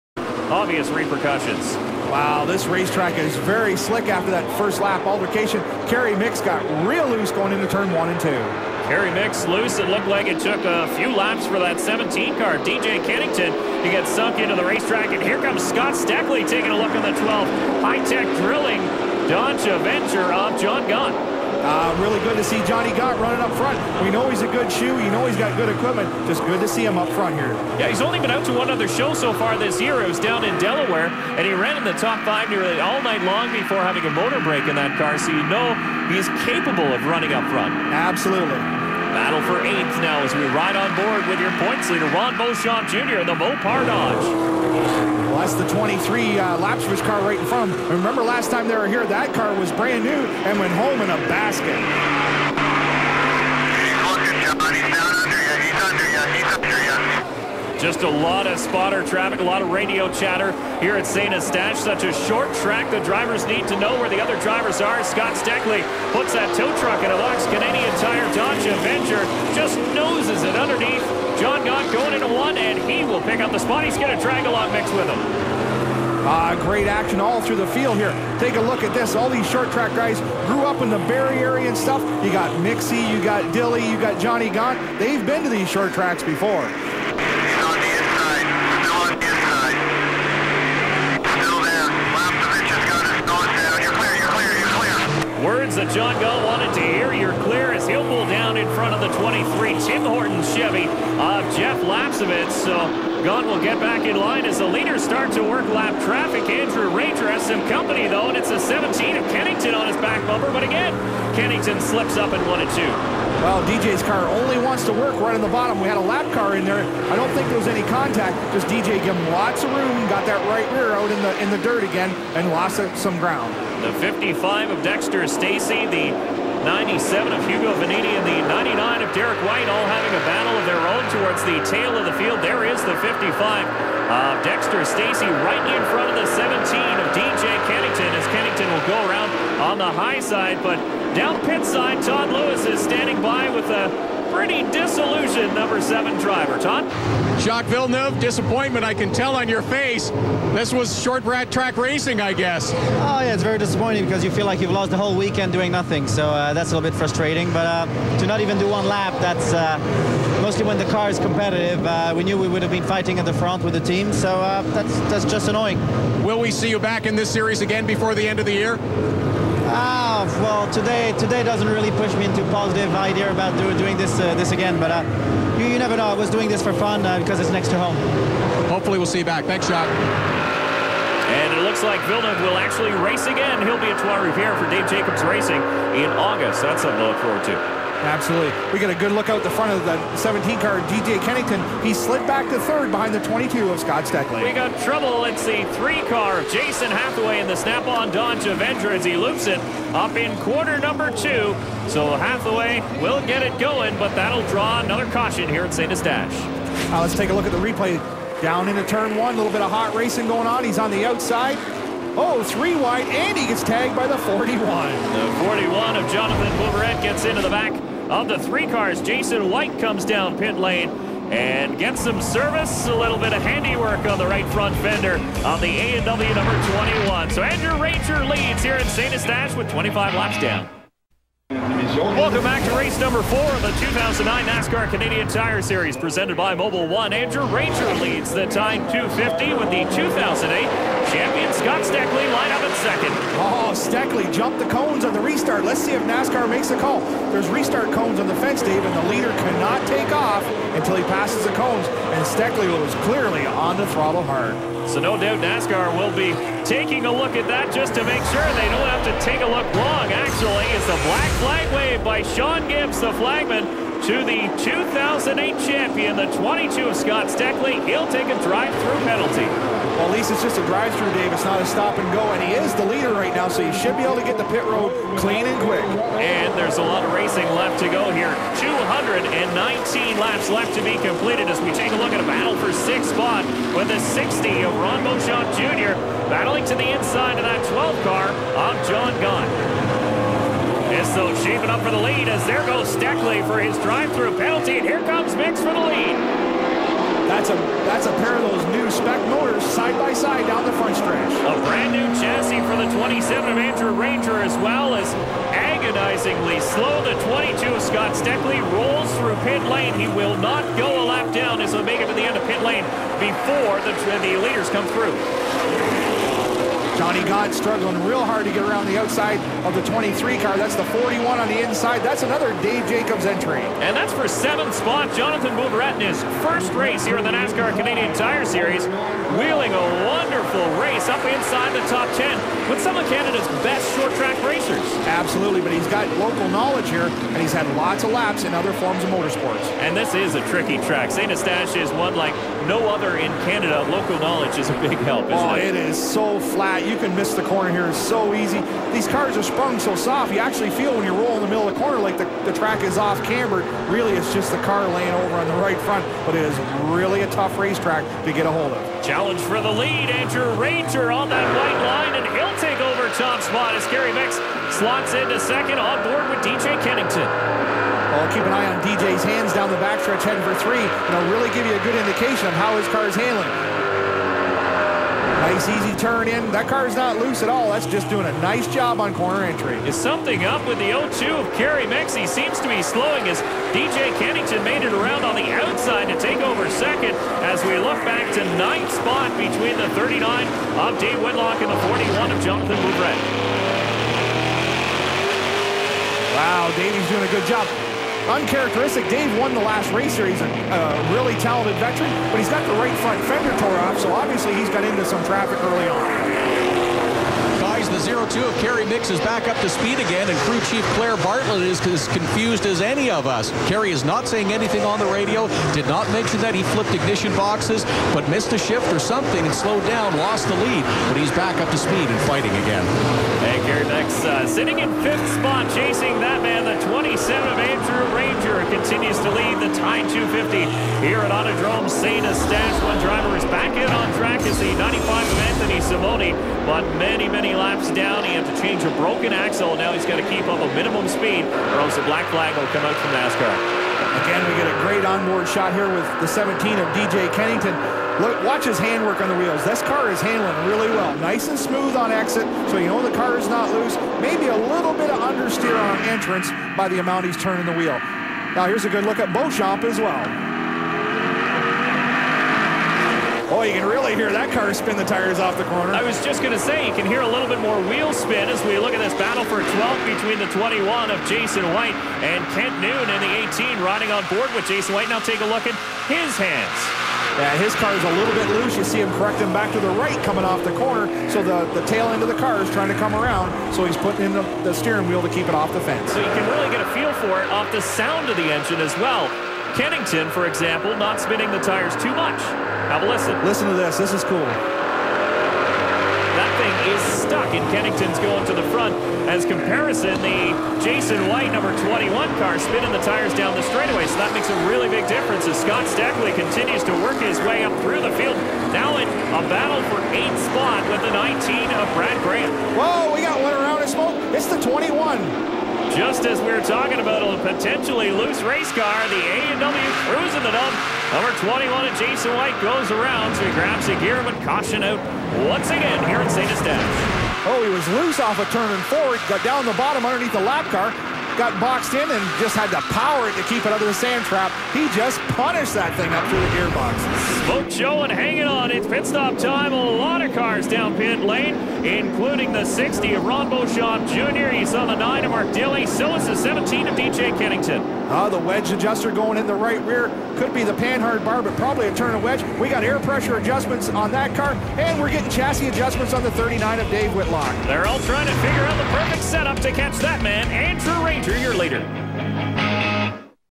2009 Tide 250 - PART 2/5
With Andrew Ranger up front there are many great battles throughout the field.  # 3 Jason Hathaway spins to bring out a caution.
On the restart Scott Steckly gets black flagged for jumping the start before #21 Jason White brings out another caution for a spin in turn 2 as we knock off the first 50 laps.

2009 Tide 250
Autodrome St.Eustache